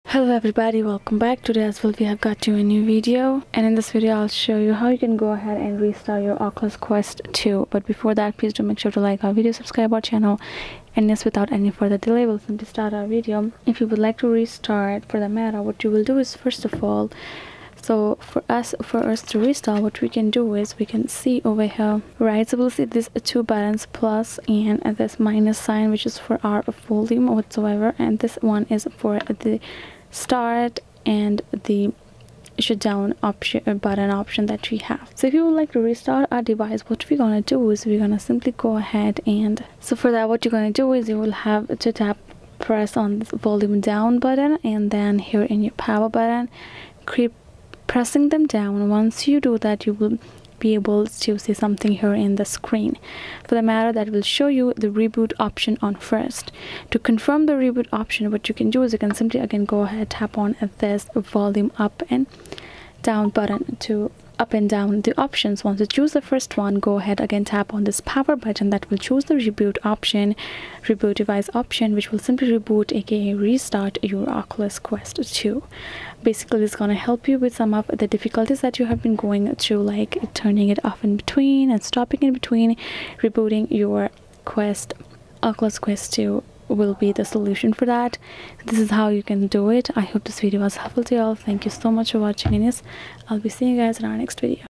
How to Restart Oculus Quest 2 | 2023
In this tutorial, I will guide you on how you can easily Restart Oculus quest 2 by following some easy steps.
0:00 Intro
0:13 Restart
1:27 Conclusion